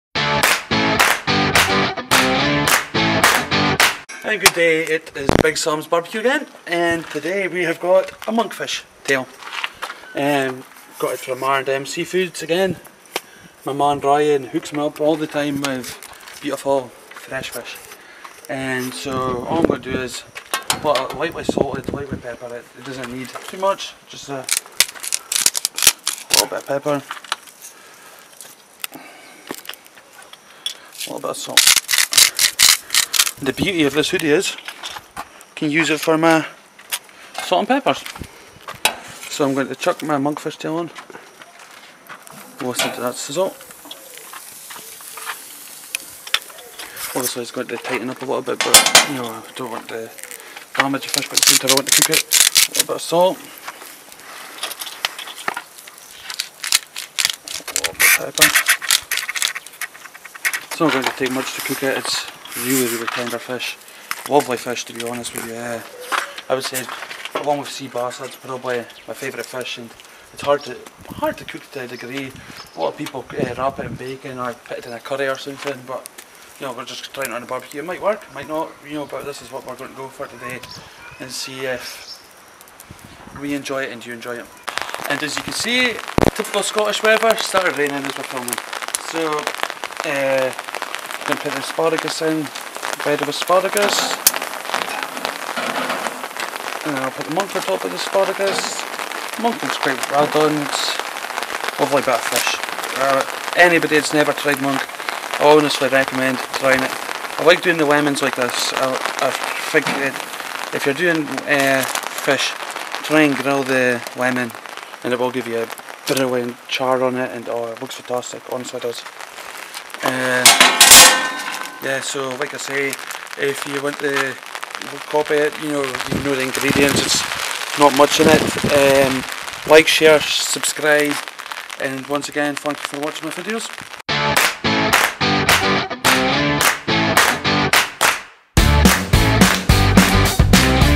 BigSam's Quick Monkfish Tail with Asparagus and Lemon
Good Evening everyone and tonight we have some monkfish tail, i hope you enjoy as much as we did and remember to like, share and subscribe and turn the bell on for post notifications! Also follow our social medias for updates and sneaky peaks!